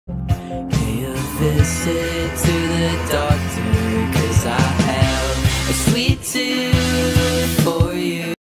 Sweet Tooth wip
Finally something that I haven't cancelled, this is too cute to stop working on.
Originally was going to be the first introduction for Token(+ Clip) because I wanted it to just be them being cute, but sadly I finished his current first appearance before. Either way it's cute though, I love these guys.

Again, sorry for all the posts.
I'll unlist this one anways. Just for a few days.

- I forgot to un-unlist it, sorry,

I had a whole week to catch any spelling mistakes and I still used to instead of too.